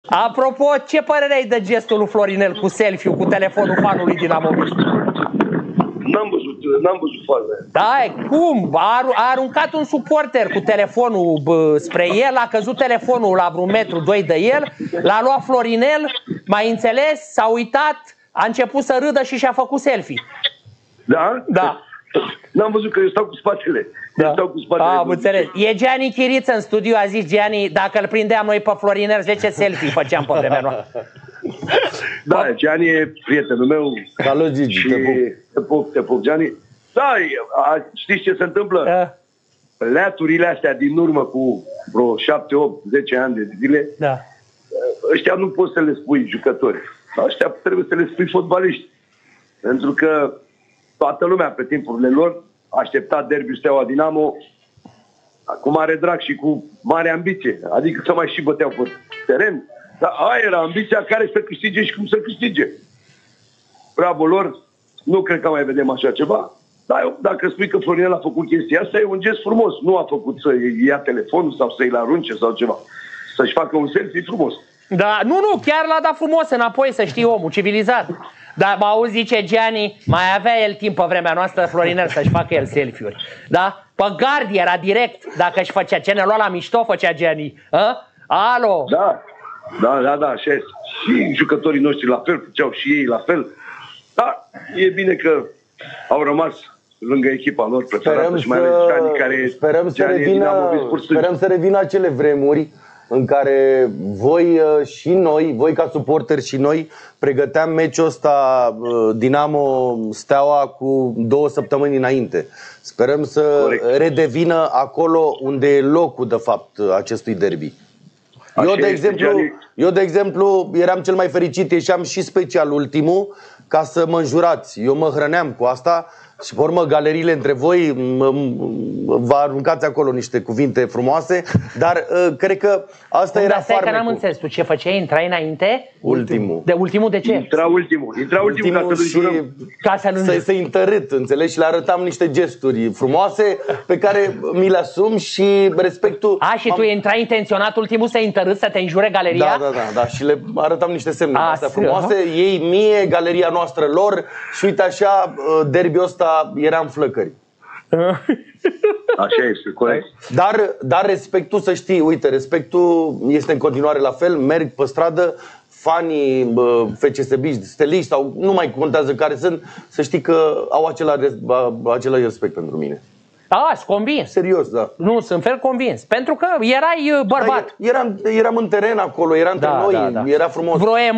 Giani Kirita - Gigi Mustata, DIALOG SPUMOS | RIVALITATEA FCSB - Dinamo
Giani Kirita si Gigi Mustat au avut un dialog spumos la FANATIK SuperLiga cu Horia Ivanovici dupa FCSB - Dinamo 2-1. Ce au spus Giani Kirita si Gigi Mustata despre rivalitatea dintre FCSB si Dinamo.